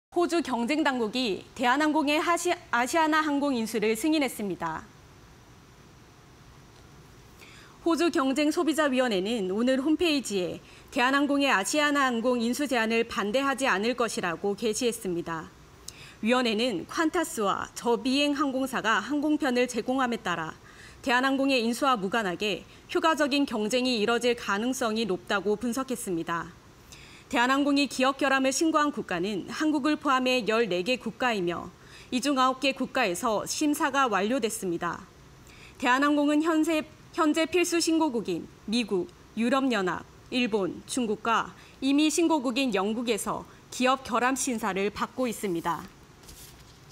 호주, 대한항공·아시아나 합병 승인... "효과적 경쟁될 것" (20220901)
호주, 대한항공·아시아나 합병 승인... "효과적 경쟁될 것"